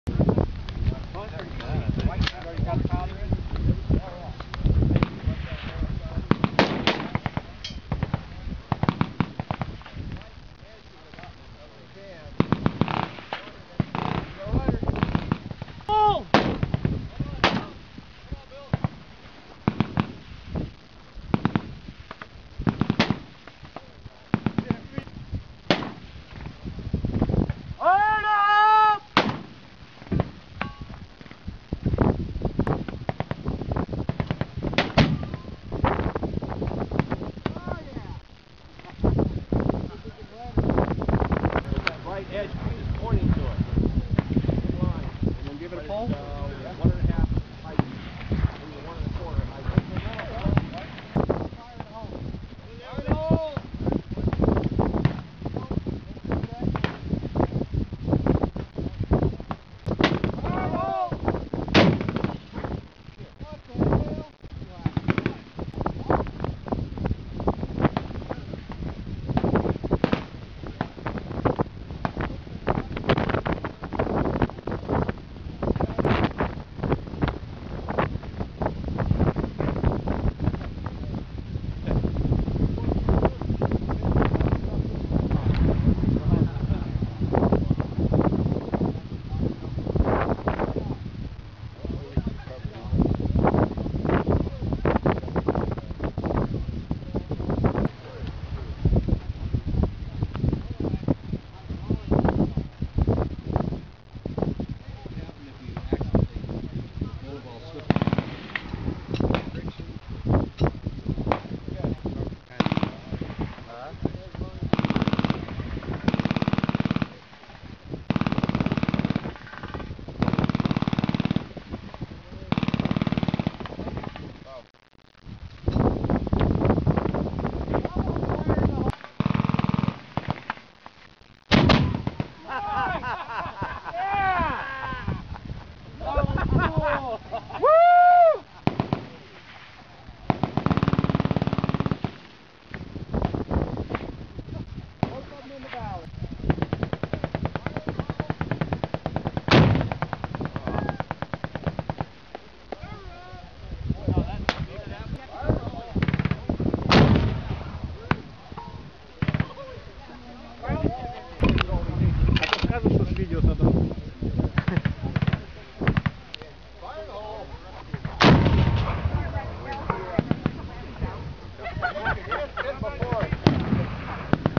The Big Sandy Shoot - March 2010 - Cannon shooting
Сходка аризонских пулемётчиков